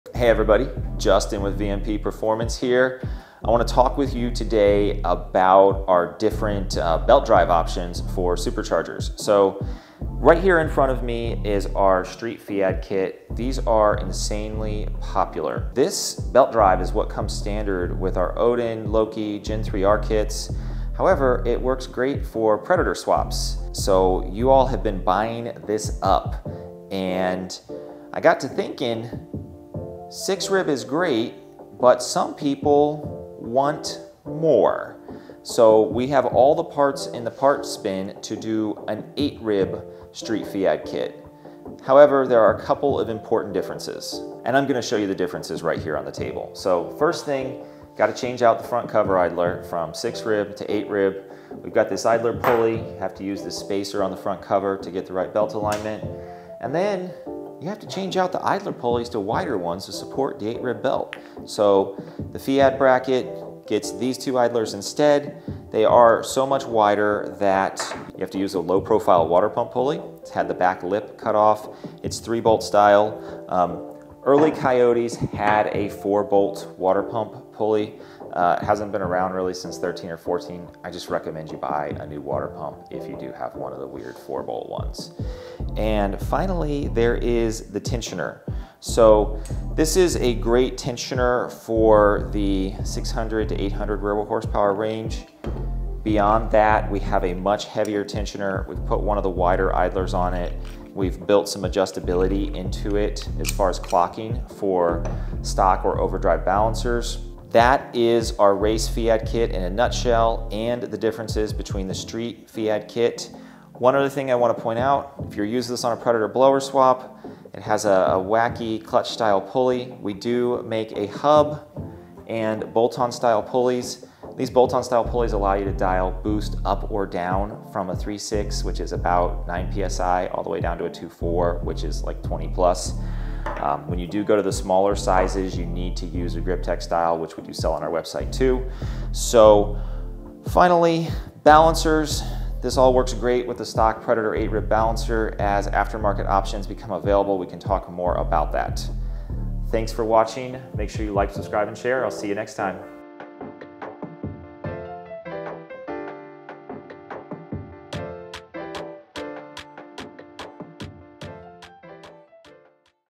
Get the Most Out of Your Supercharger with VMP's 8 Rib FEAD Kit!
Today, Justin is diving into our belt drive options for superchargers, specifically designed for those looking to upgrade or modify their setups. We’re showcasing our popular Street FEAD kit, standard on our Odin, Loki, and Gen 3R supercharger kits, as well as an 8-rib upgrade for extra strength and durability. These kits are also great for Predator swaps!

In this video, Justin breaks down the key components and differences between the Street and Race FEAD kits, covering essentials like the front cover idler, low-profile water pump pulley, and belt alignment adjustments. He also explains the range of boost options available with our bolt-on style pulleys, which allow easy tuning from about 9 PSI to 20+ PSI.

If you're interested in fine-tuning your setup for more boost, improved belt stability, or compatibility with Predator swaps, remember to check out the parts mentioned in the video on our website and don’t forget to like, subscribe, and share!

► VMP Street No-Grind 8-Rib FEAD for Supercharged Coyote 5.0 L